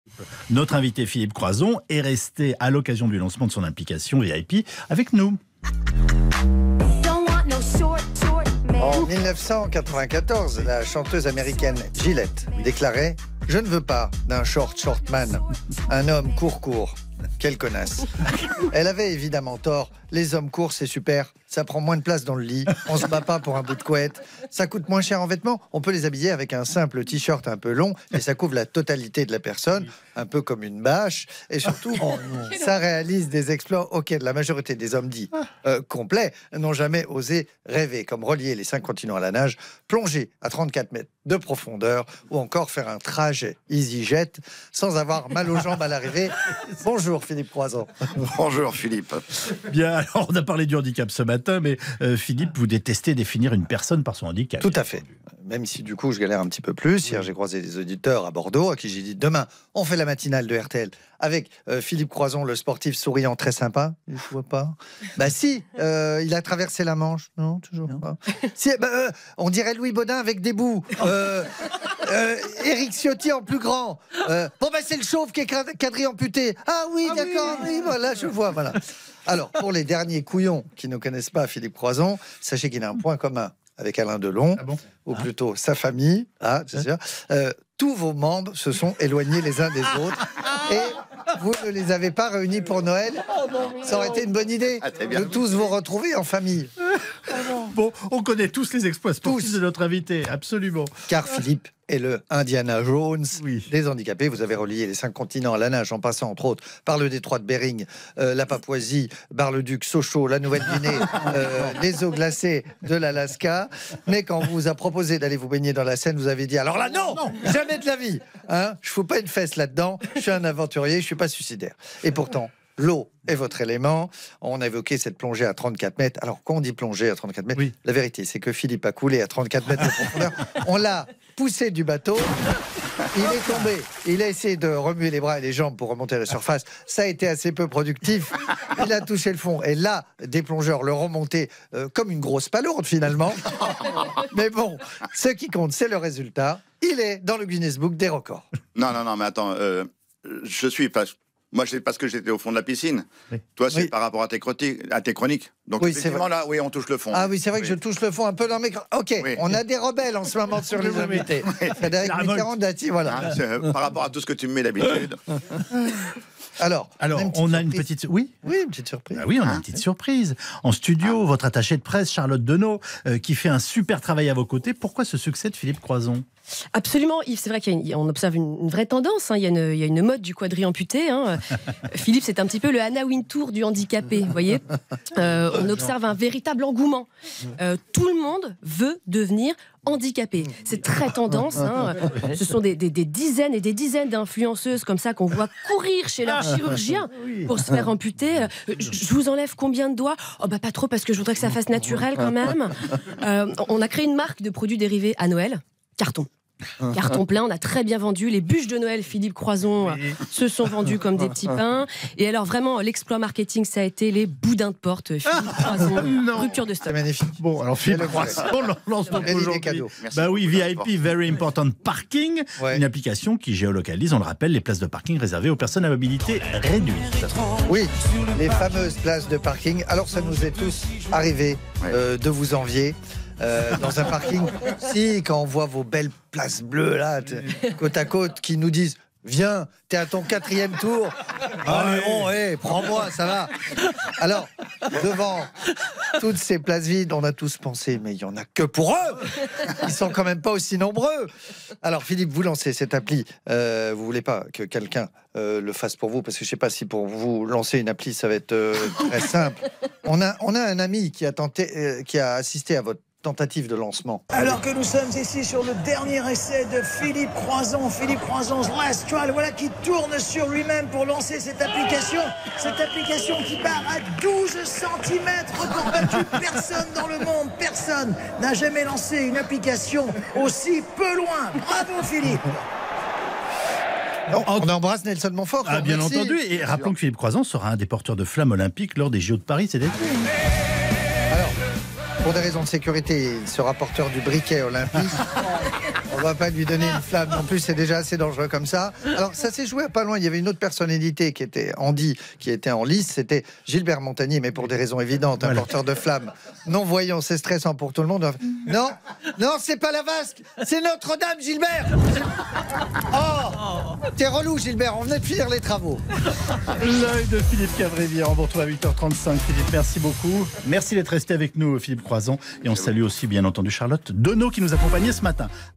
Philippe Caverivière face à Philippe Croizon
Philippe Caverivière face à Philippe Croizon : il lance une application pour aider les personnes handicapées à trouver une place de parking